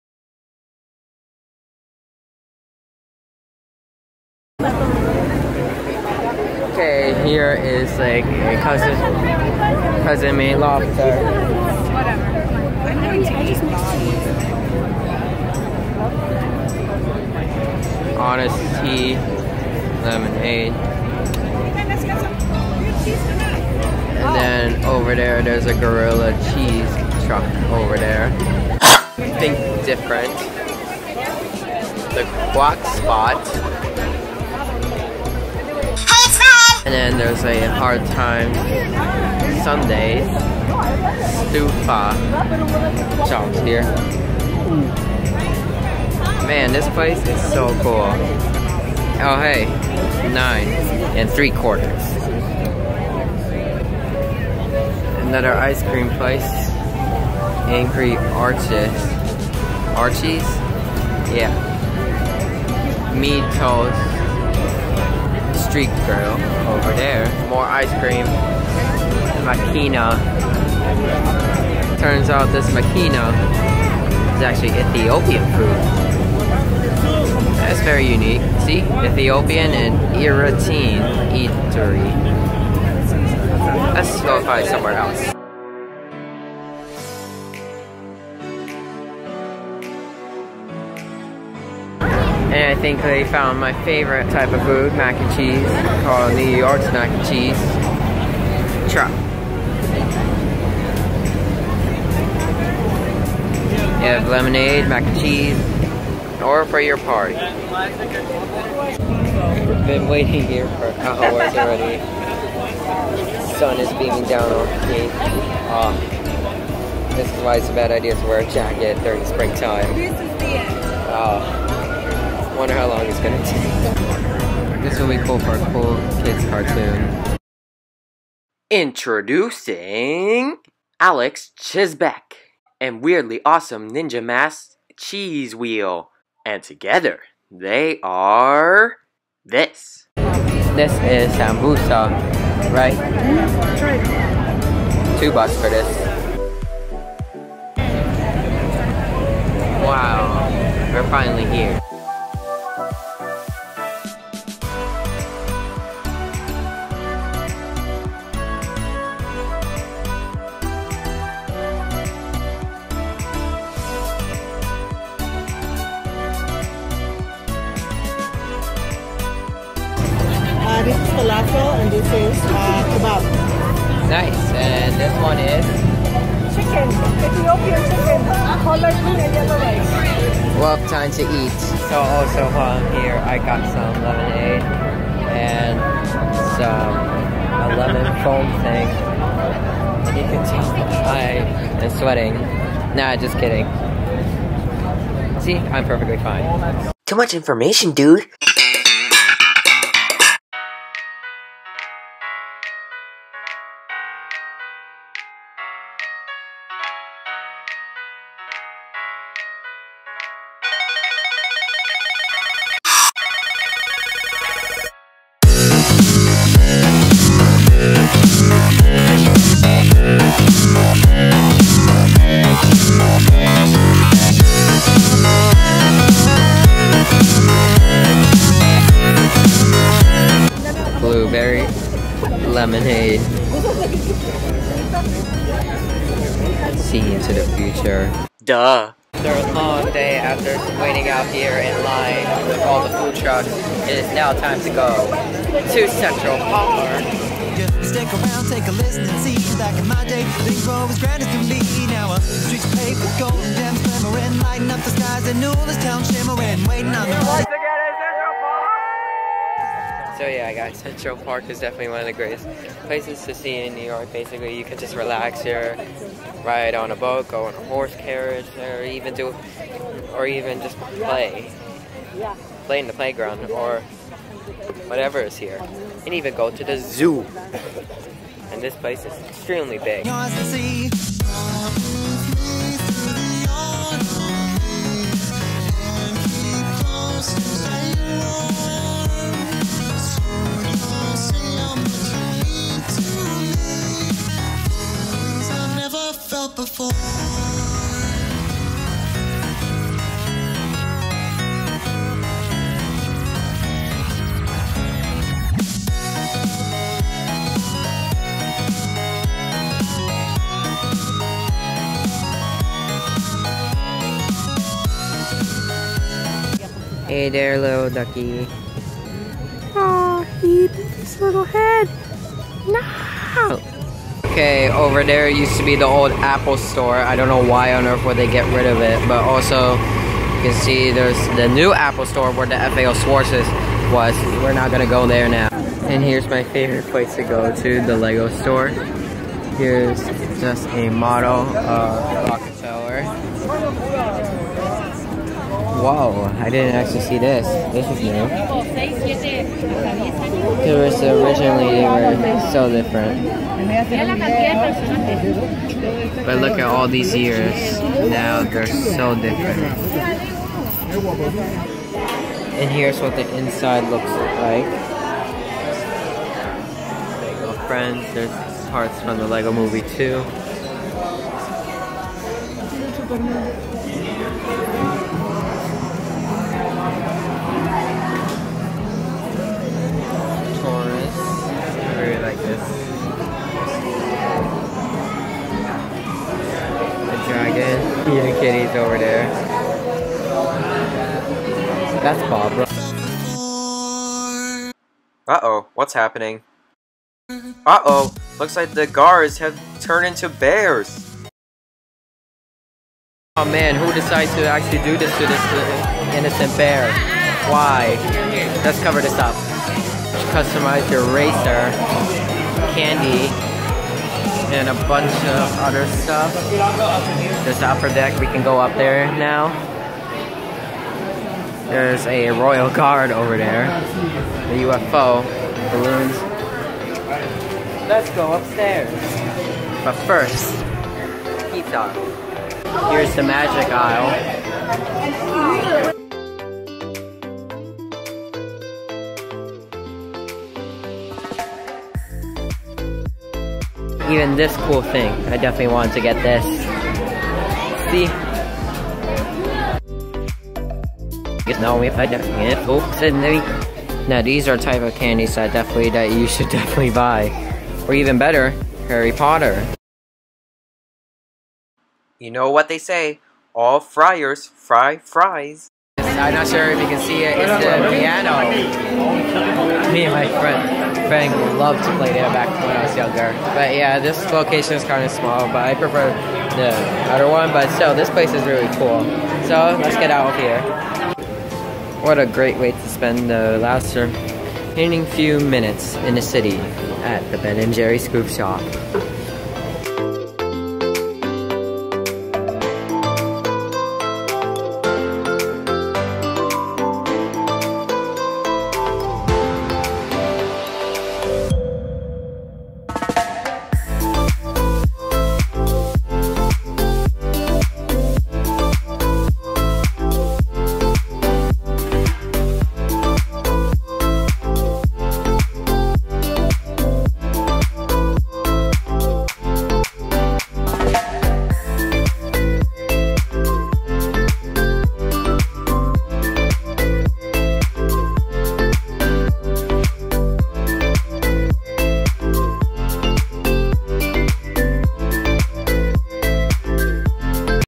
NYC Food Truck Festival-Mini Vlog
Just a simple day in NYC back in April of 2019. We were at a food truck festival, went to Central Park, and FAO Schwartz (Which reopened.)

I had a fun time. Wish we can go back to this, but that is only if we maintain our distance, wear a mask, use sanitizer etc.